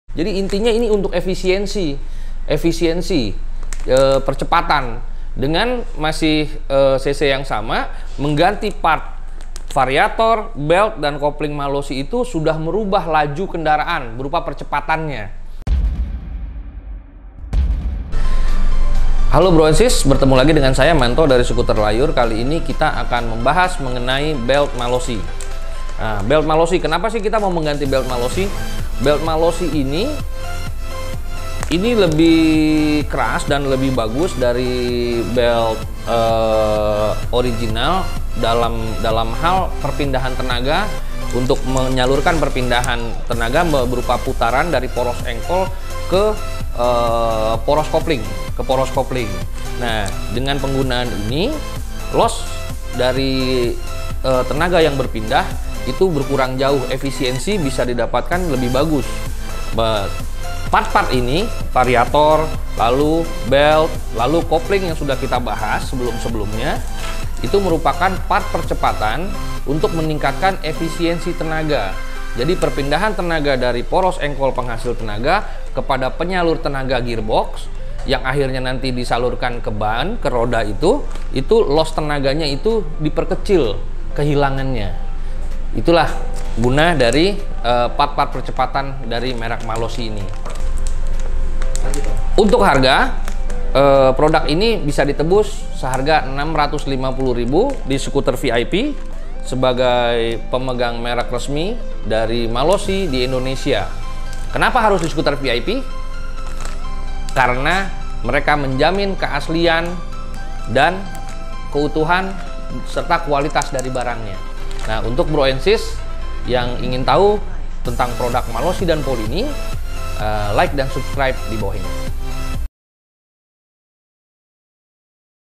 Scooter VIP : Yang bikin efisiensi percepatan buat Modern Vespa dengan Kevlar Belt Malossi
Hi BRO & SIS, selamat datang di Scooter VIP TV Channel di Youtube!

Di episode kali ini kita sharing tentang part buat Modern Vespa lagi nih, yaitu BELT MALOSSI untuk Vespa Primavera/Sprint 125ie 3V ie. Dan apa aja sih yang kalian dapetin di dalam box nya ketika beli part BELT dari MALOSSI ini? Simak episode kali ini bersama kontributor kita Pak Manto dari SCOOTER LAYUR.

Mending langsung di Play aja kali yah videonya daripada penasaran!